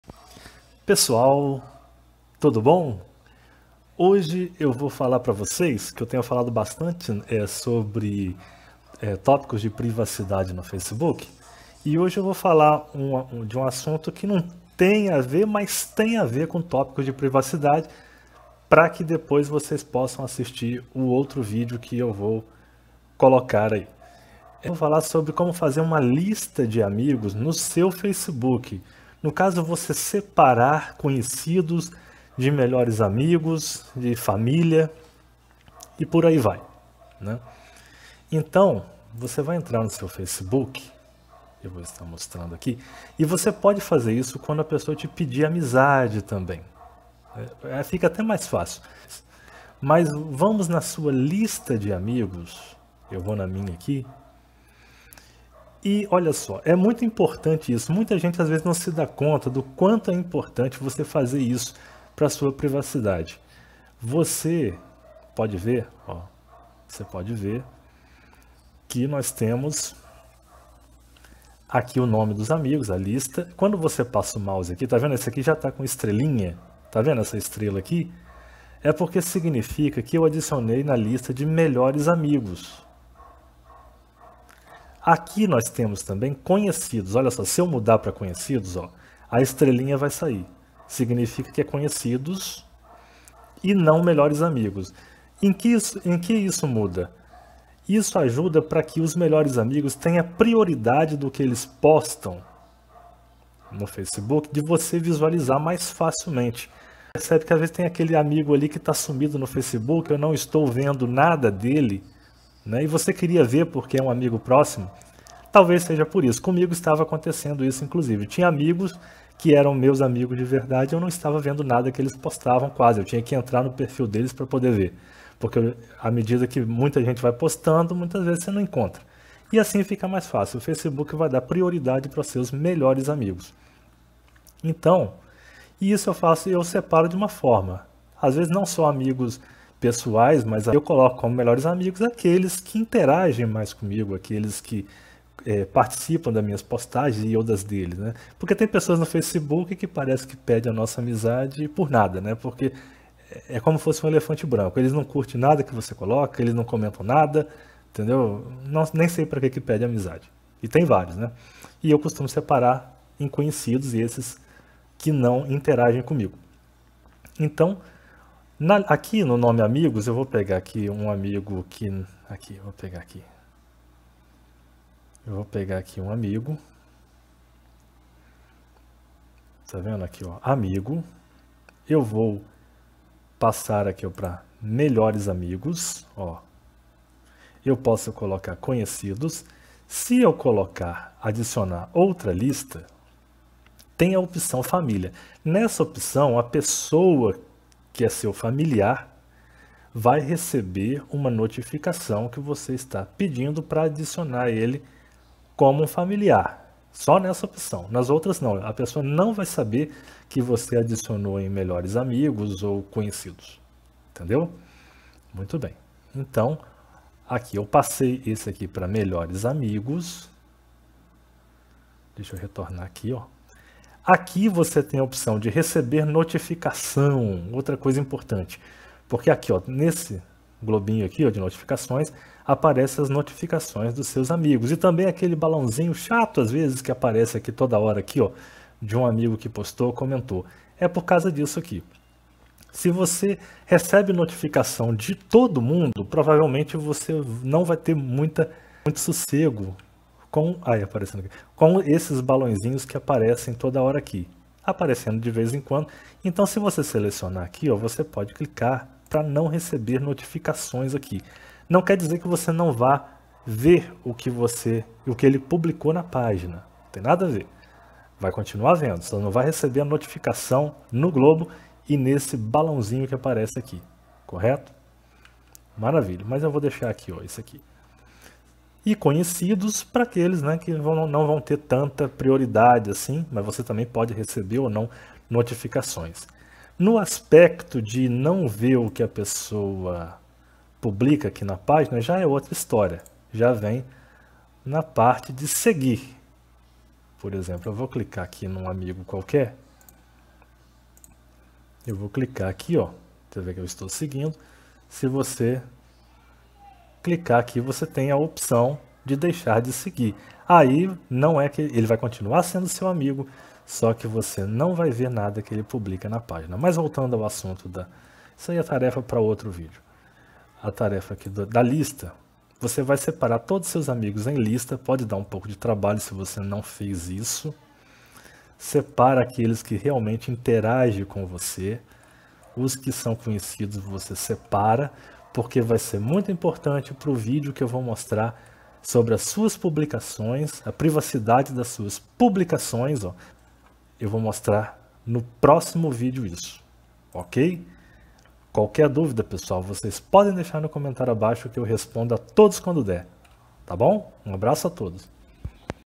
Fazendo uma lista de amigos no Facebook
Separando seus amigos do Facebook em Conhecidos, Melhores amigos e Família. E optar por receber notificações ou não de determinado amigo.